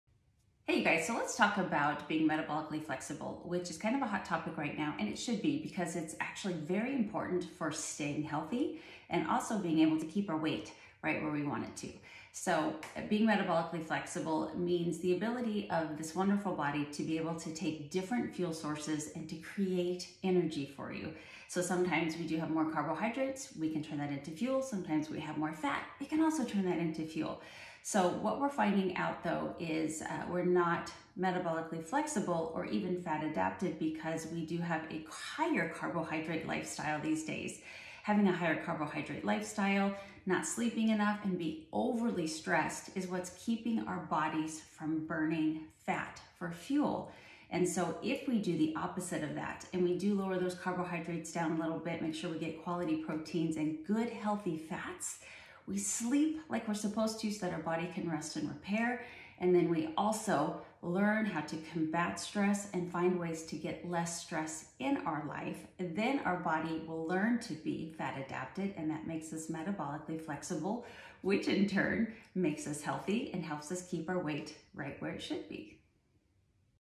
Melissa McAllister explains Metabolic Flexibility
Melissa McAllister, a functional nutritionist and trainer, explains what metabolic flexibility is and provides tips to improve metabolic flexibility.